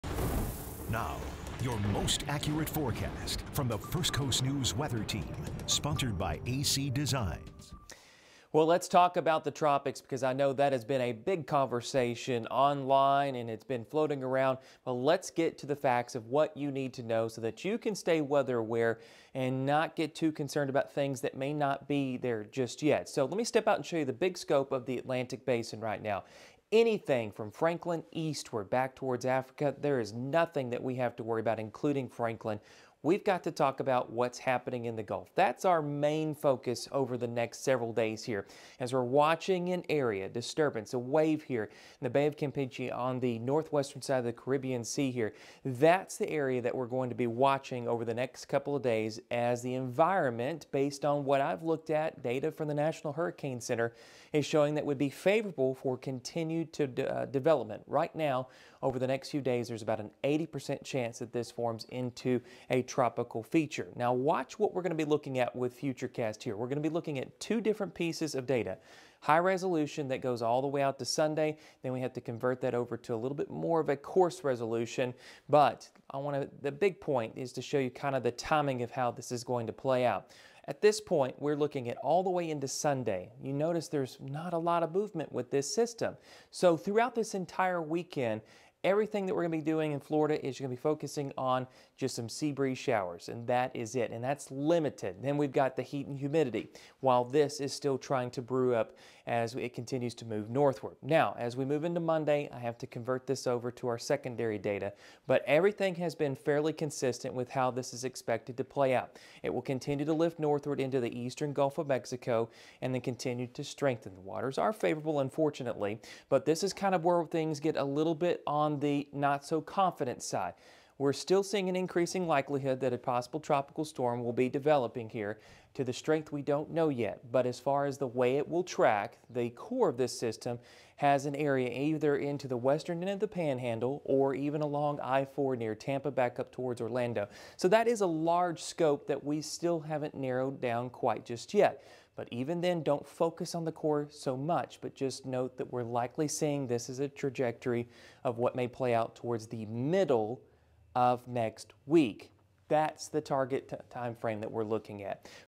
Hurricane Tracking: Activity in the Gulf of Mexico has '80 percent' of developing into tropical syst
A possible tropical system has a chance at forming over the next few days in the Gulf of Mexico.  There is currently no threat posed to residents on the First Coast.